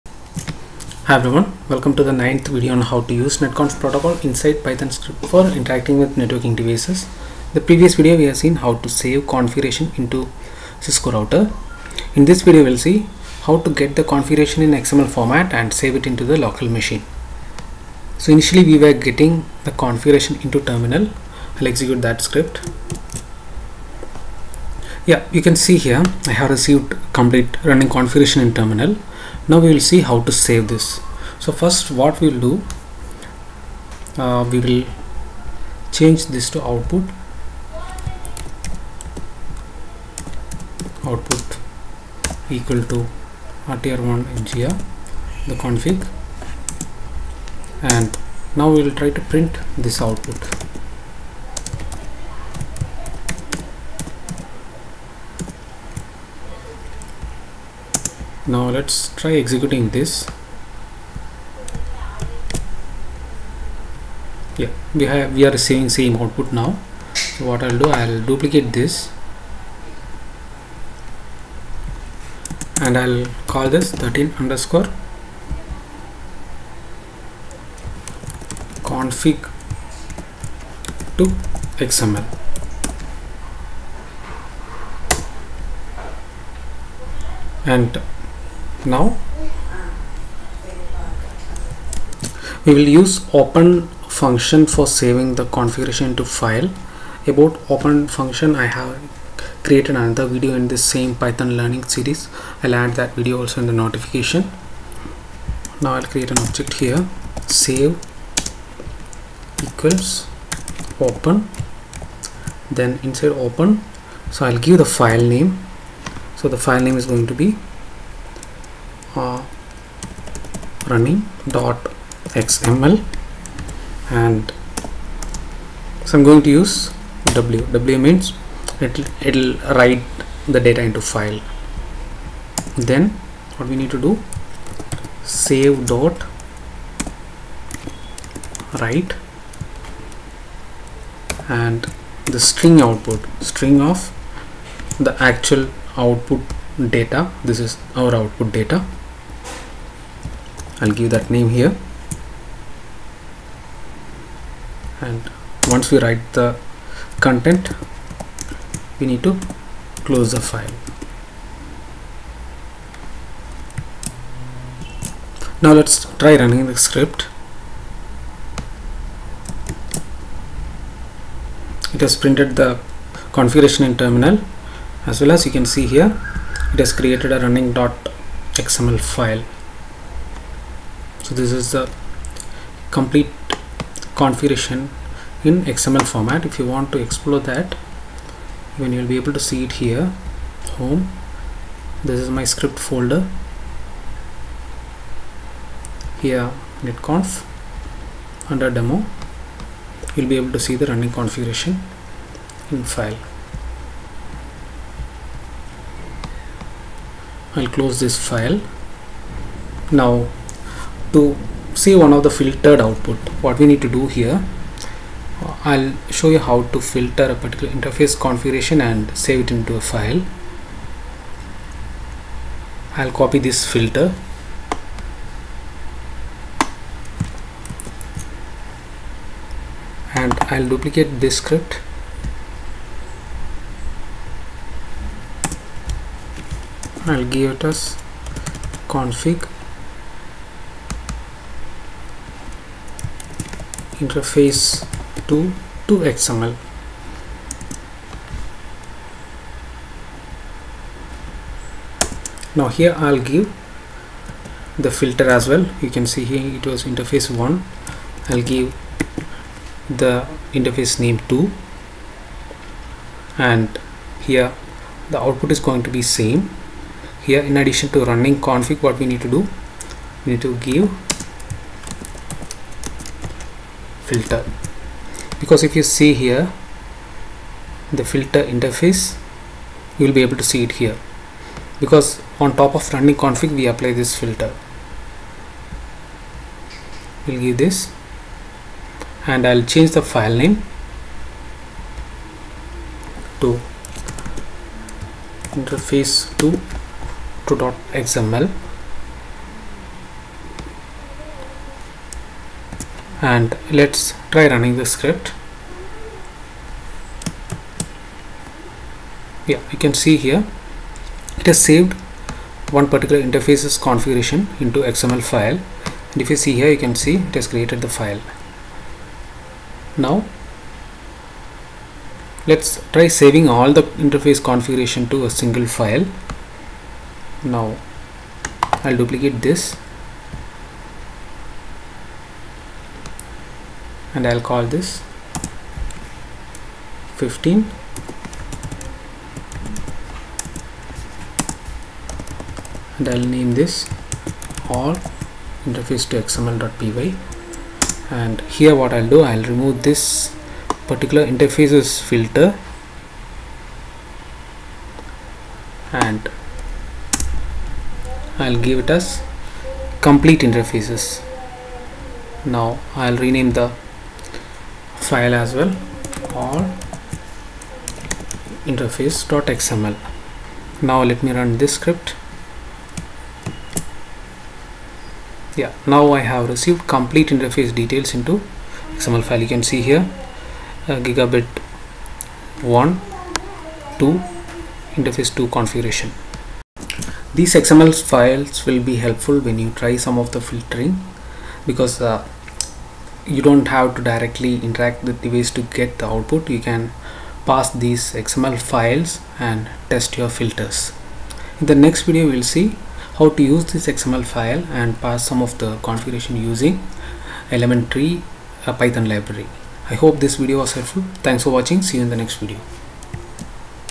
NETCONF Python Example | Part 9 | Save Cisco Configuration in XML File
This video demonstrates how to save Cisco IOS configuration in xml format using Python
How to use netconf protocol for configuring devices
How to filter interface configuration using Python
How to use nccclient python library to automate cisco configuration
Tutorial on using xml for configuring cisco devices